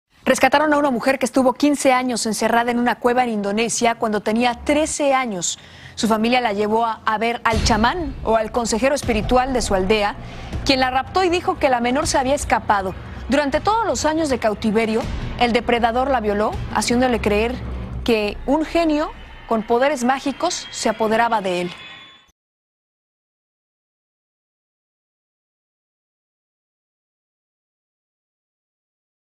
Rescatan a una joven que estuvo 15 años retenida en una cueva por un chamán
El incidente ocurrió en Indonesia y, según las autoridades, durante los años de cautiverio el depredador la violó y la maltrató.